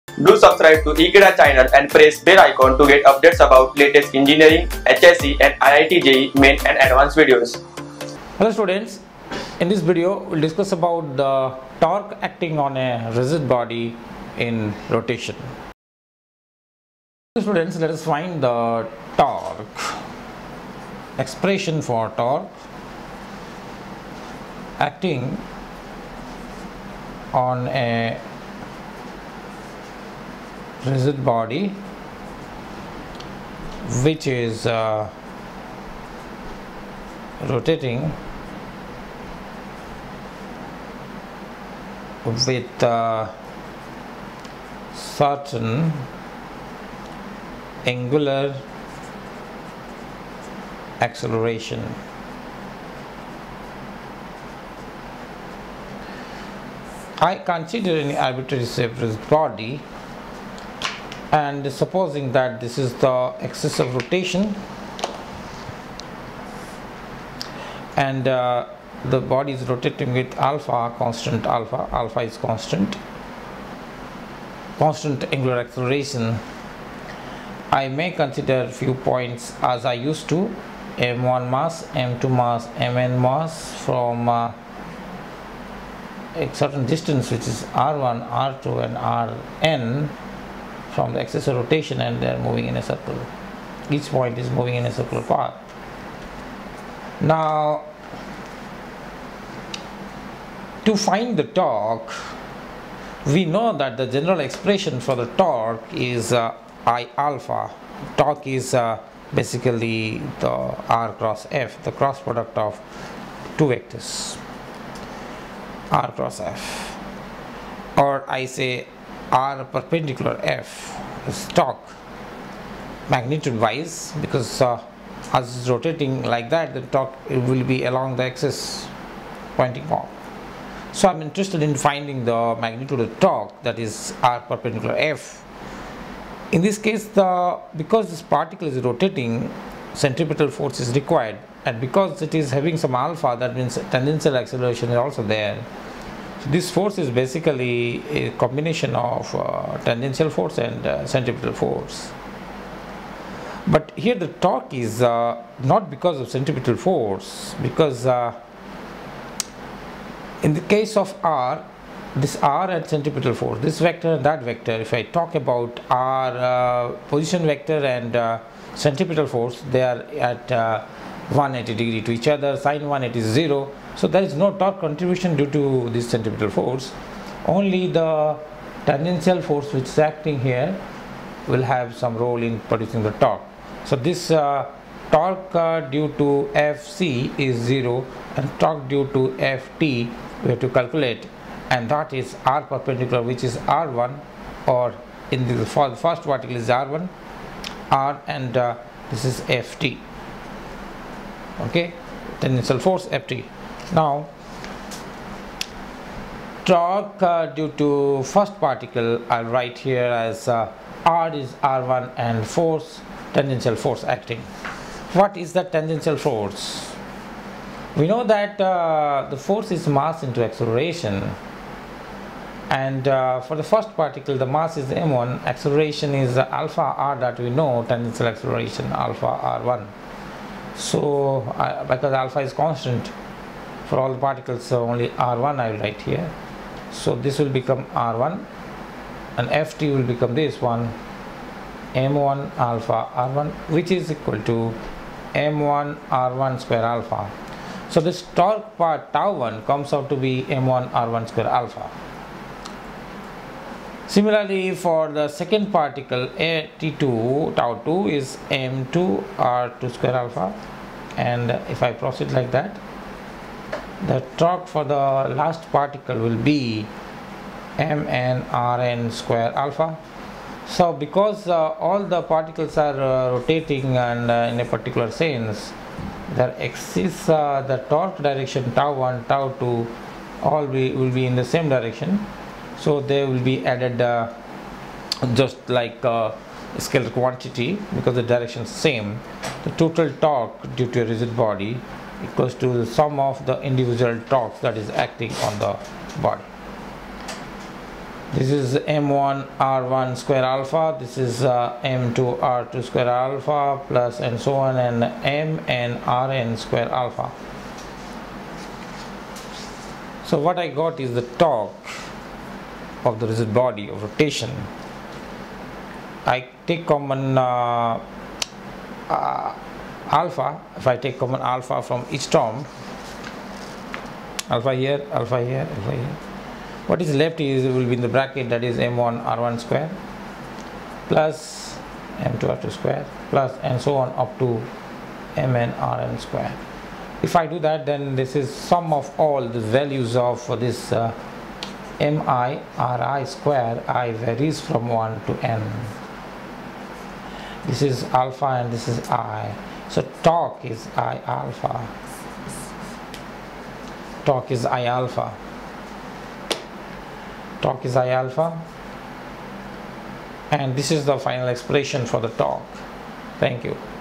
Torque Acting on a Rigid Body in Rotation - Rotational Motion - Physics Class 12
Torque Acting on a Rigid Body in Rotation Video Lecture from Rotational Motion chapter of Physics Class 12 for HSC, IIT JEE, CBSE & NEET.

Watch Previous videos of Chapter Rotational Motion:-

Happy Learning : )